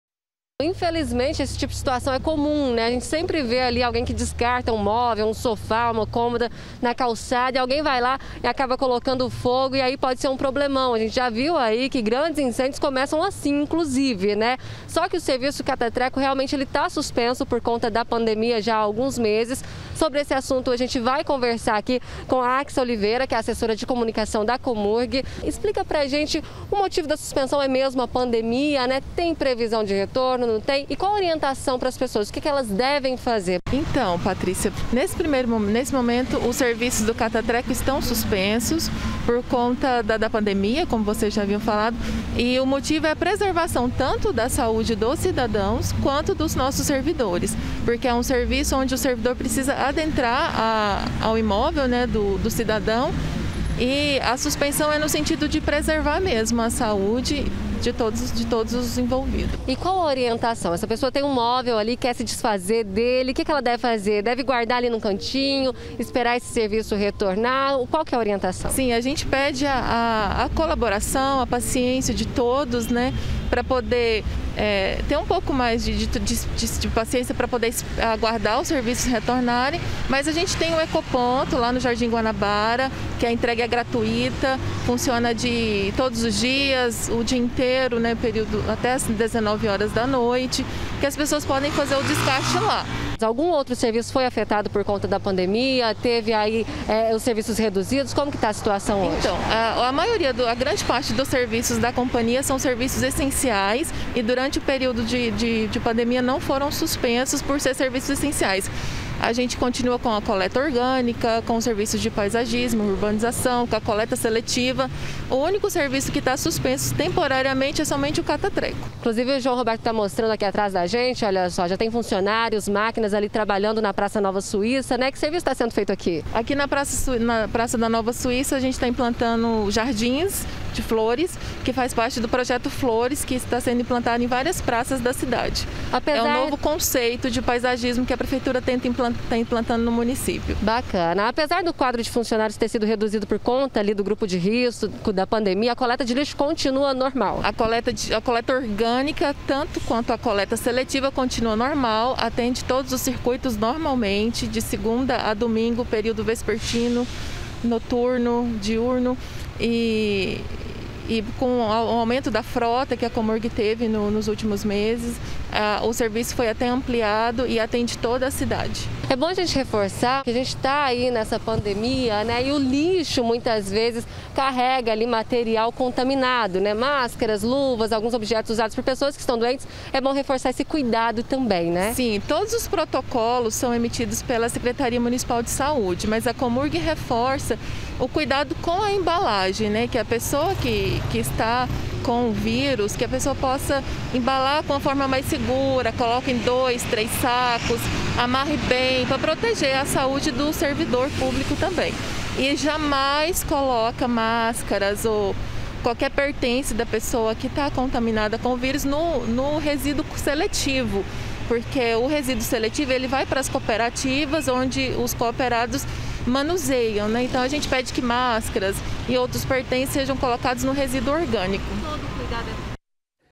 CATA-TRECO ESTÁ SUSPENSO POR CONTA DA PANDEMIA
ALGUNS SERVIÇOS DA COMURG ESTÃO SUSPENSOS POR CONTA DA PANDEMIA. UM DELES É O CATA TRECO. MAS O QUE A POPULAÇÃO PODE FAZER COM MÓVEIS QUE PRECISAM SER DESCARTADOS ATÉ LÁ.